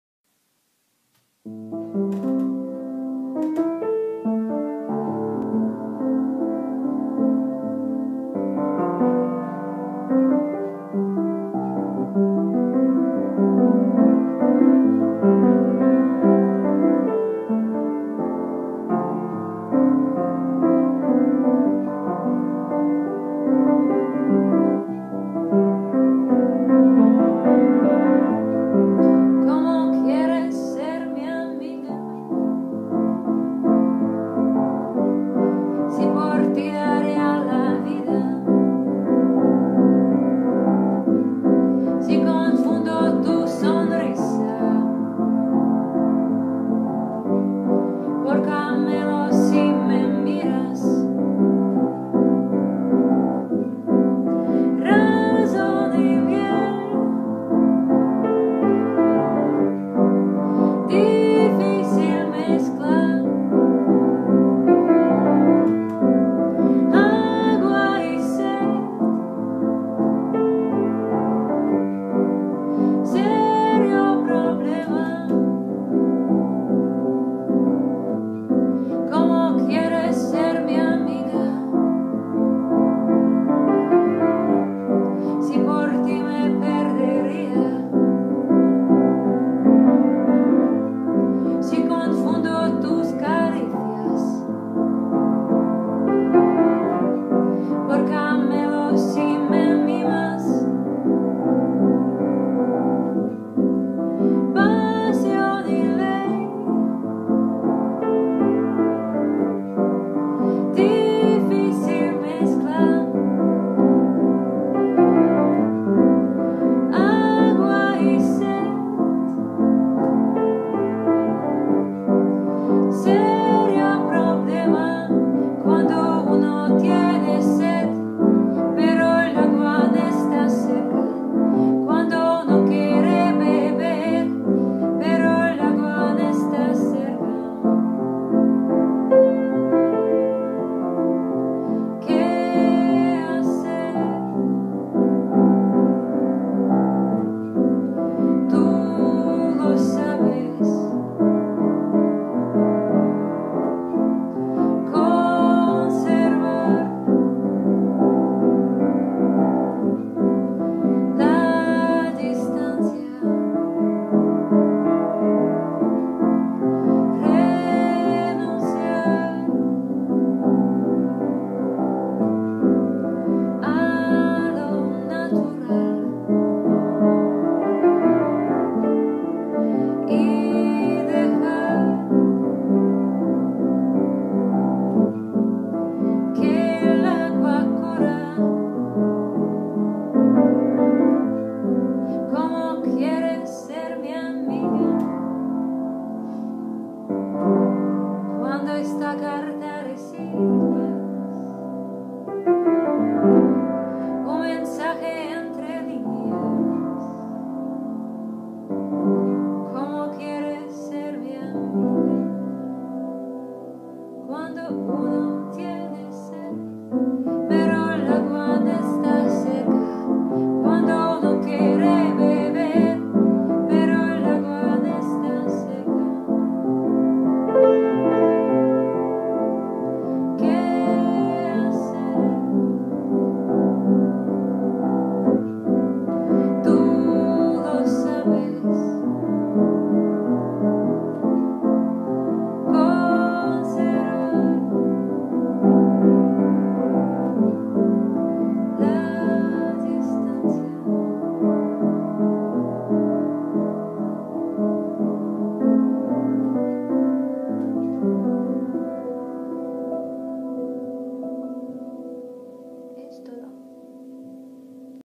Spanish courses Taronja Darya
Nuestra alumna Darya aprendió una canción de Jarabe de Palo en su clase de español. Nos ha enviado este vídeo donde canta la canción tocando el piano. Muchas gracias Darya  : )))

Spanish courses in Valencia, Escuela Taronja